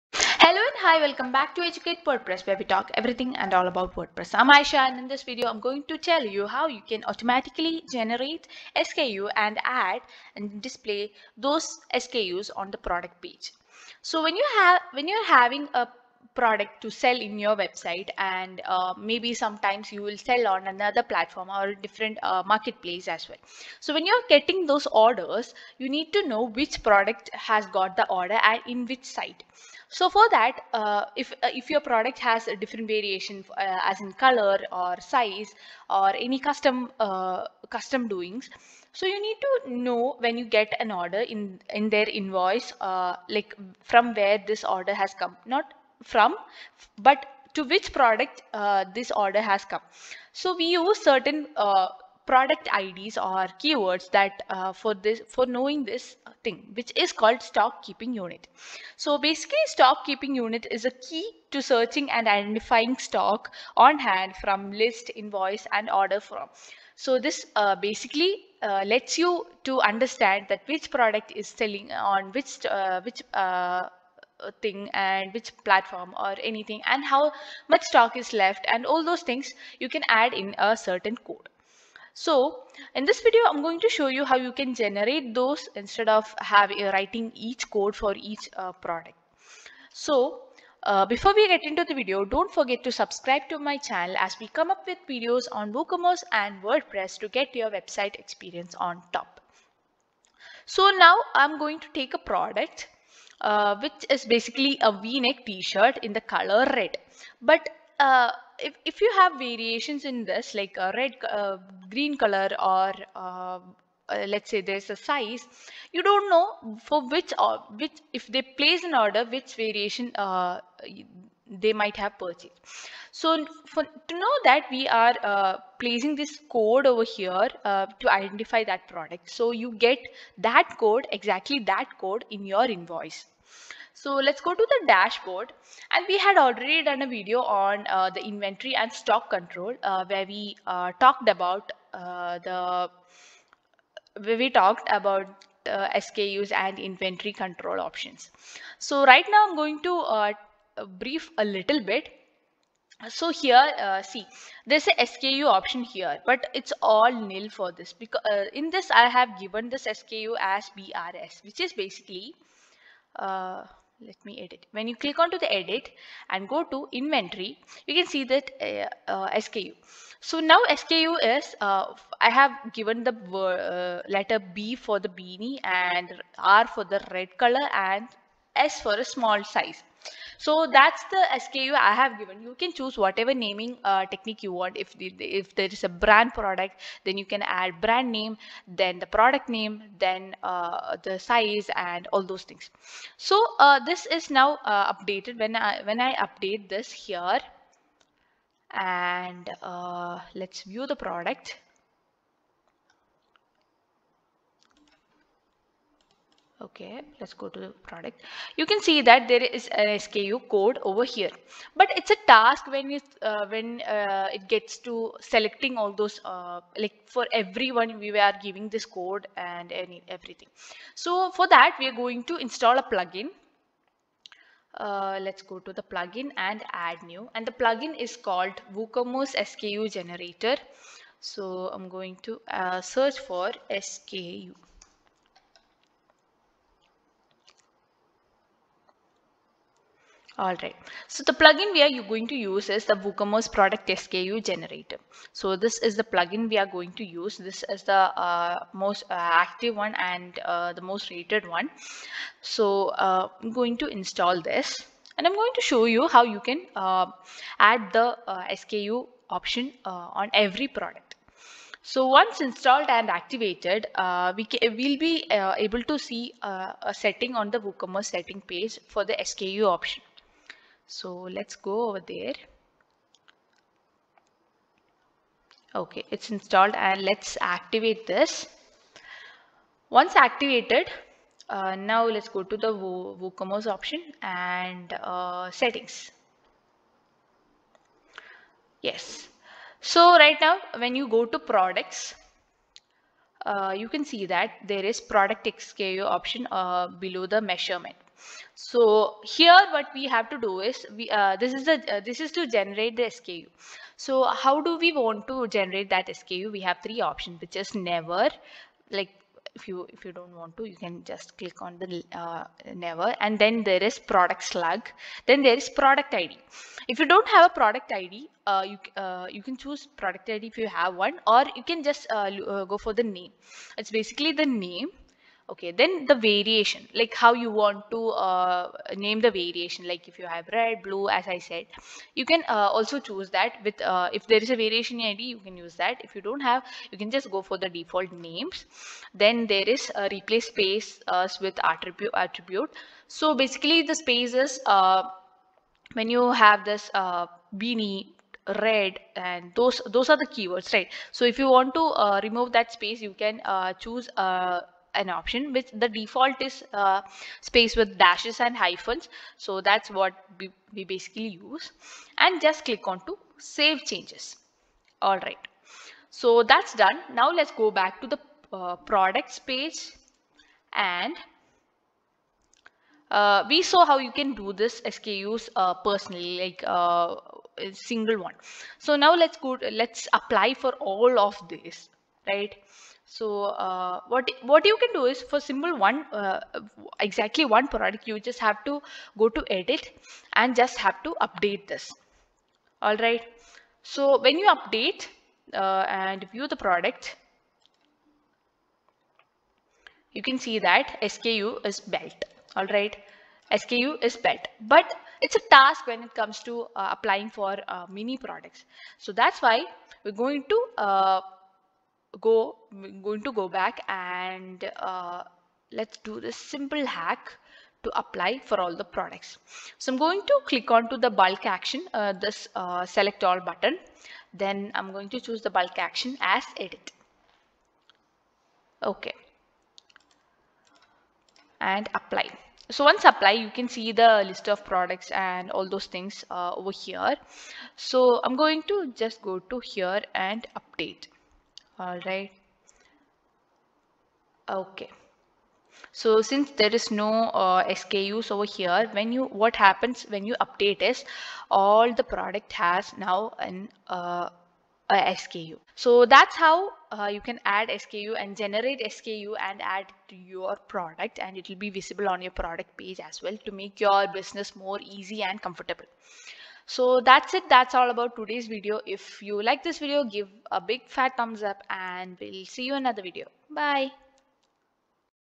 Automatically Generate SKU & Add and Display it on product page of WooCommerce Website | WordPress
What is SKU? Why is it important for your online e-commerce stores? How can you automatically generate SKU for market place? How it displays on the product page and how can you manage the stock. Watch the video for a better understanding of the stock-keeping unit. Using an SKU generator and improving your SKU generation process early on reduces the potential for errors.

Automatically generate an SKU for parent / simple products, variations, or both when the product is published or updated. SKUs for simple, external, or parent products can be generated from the product’s slug (hyphenated name) or ID. For example, a product with the slug wp-tee-shirt can have an SKU of wp-tee-shirt or 192 (whatever the product’s ID is). This SKU will be generated any time the product is published or updated.

00:00 Introduction
00:58 What is SKU
01:40 Why SKU is needed?
02:46 How to name SKU
03:28 Why do we need to generate SKU
03:40 Install Woocommerce SKU generator
04:42 Setting up SKU
06:07 Generating SKU for each product